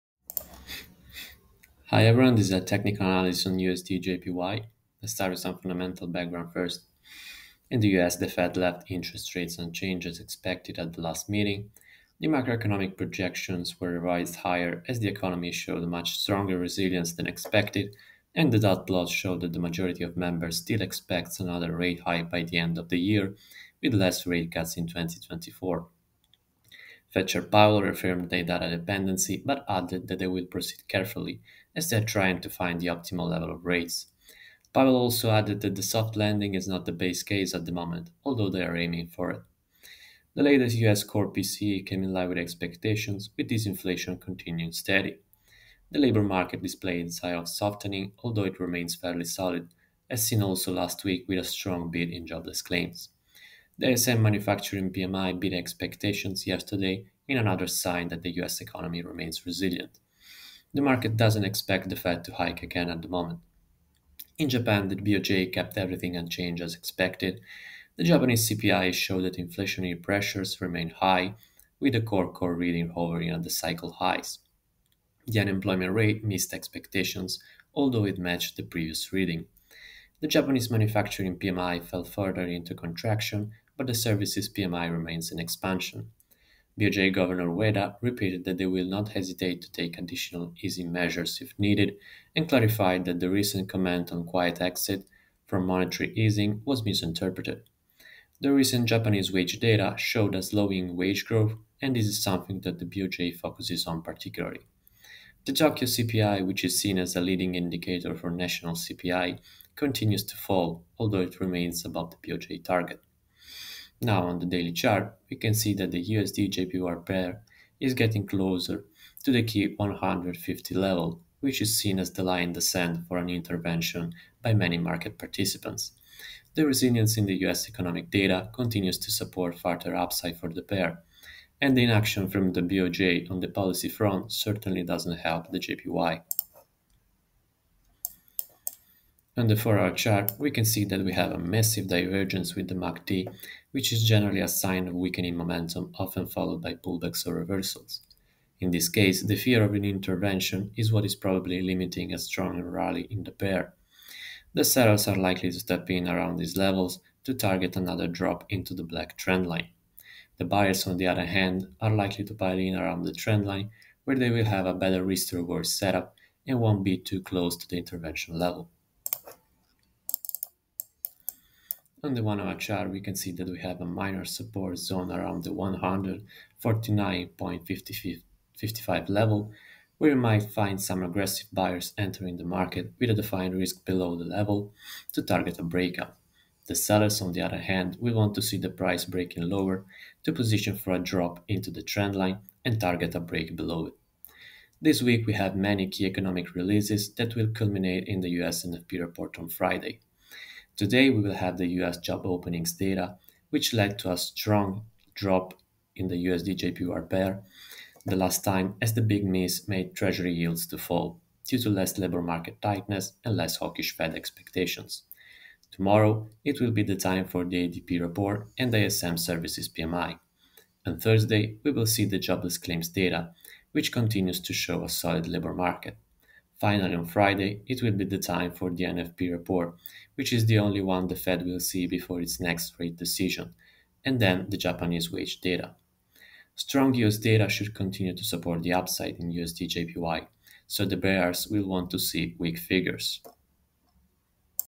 USDJPY Technical Analysis
Here's a quick technical analysis on USDJPY with some fundamental background.

For more visit ForexLive.com